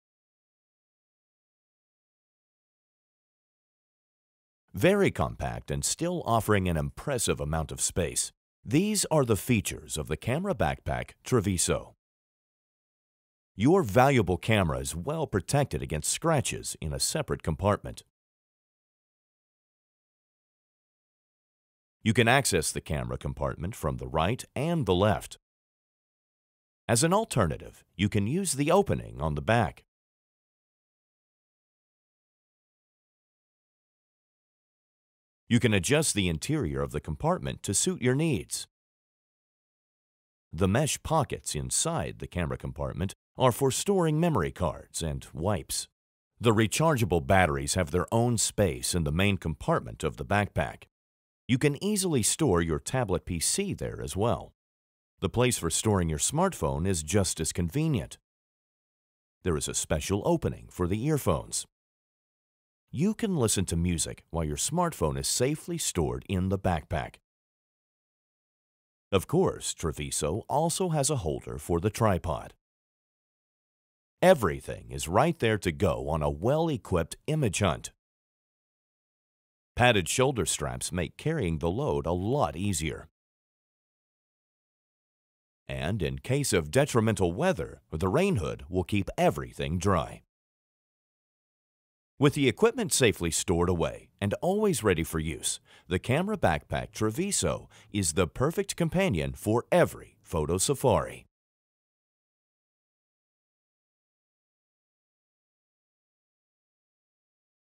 Hama Treviso Camera Backpack
You go on tour to make some impressive and stunning photos? The Treviso camera backpack is a perfect, stylish and compact companion that gives your equipment a fixed place. It permits you to have everything with you and within easy reach. What is more, it can comfortably be carried on the shoulder.